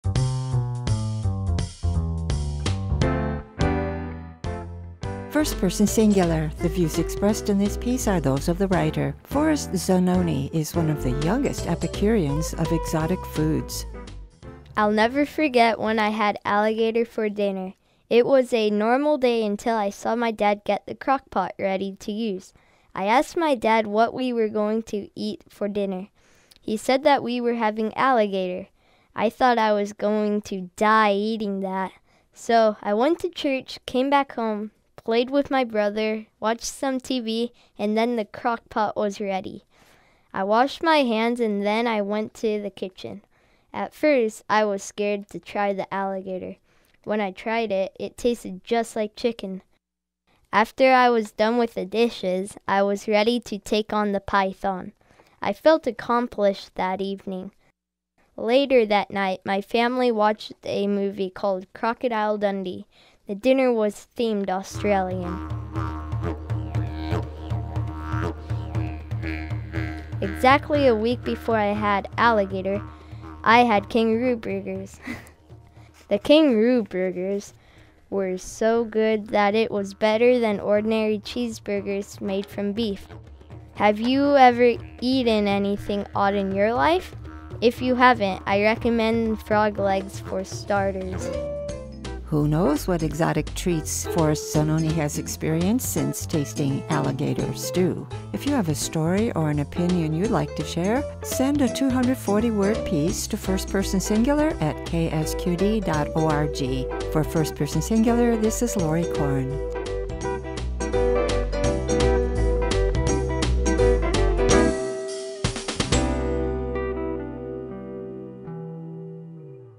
Forrest Zenone – Alligator Stew
Forrest Zenone is one of the youngest epicurians of exotic foods. Photo courtesy of SenuScape.